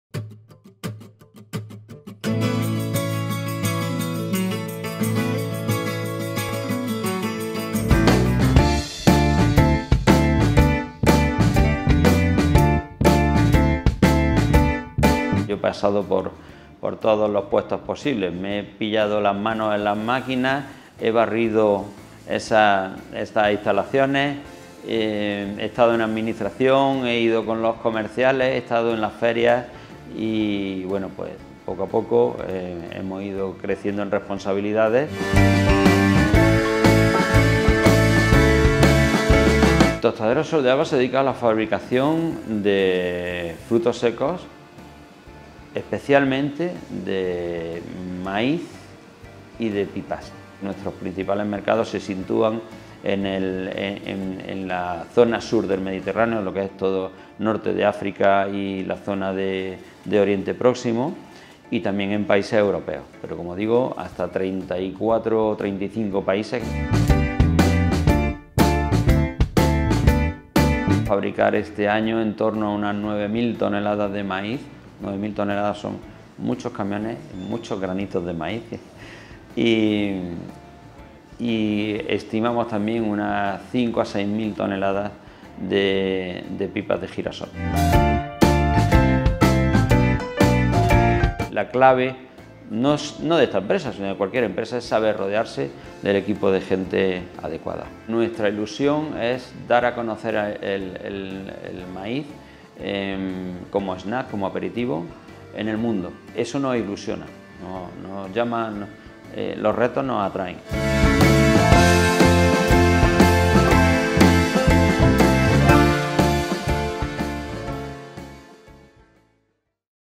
HDL José Antonio Barragán, frutos secos con sabor internacional
El padre de José Antonio Barragán decidió volver a su Granada natal desde Madrid y creó una empresa de distribución de frutos secos. Años después se convirtieron en fabricantes, y así nació Tostaderos Sol de Alba, que ahora dirige José Antonio junto a su hermano. Especializados en pipas de girasol y maíz, distribuyen por países de todo el mundo. De su fábrica granadina salen marcas como pipas 'Dakota' o el maíz 'X-Korn'.